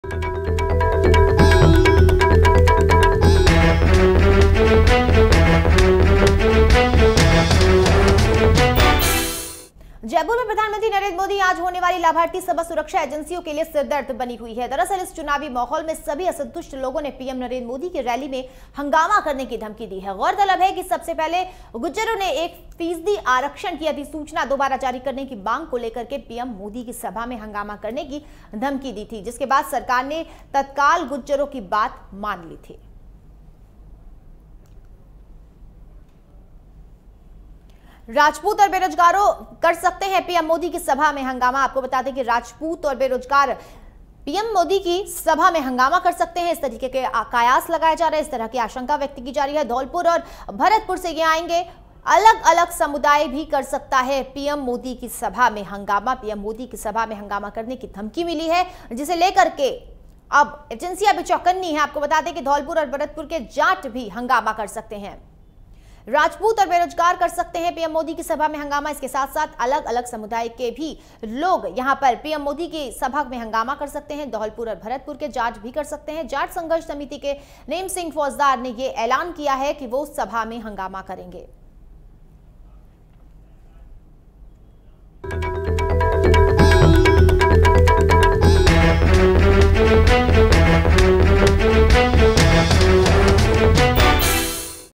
7 जुलाई को PM Modi का Jaipur दौरा, जानिए किसने दी दौरे से पहले धमकी
पीएम मोदी अब 7 जुलाई को पीएम मोदी का दौरा करेंगे।पीएम मोदी अमरुदों के बाग से जनसभा को संबोधित करेंगे।इस दौरान पीएम लाभार्थी सभा करेंगे।पीएम मोदी की सभा में हंगामा करने की धमकी मिली है।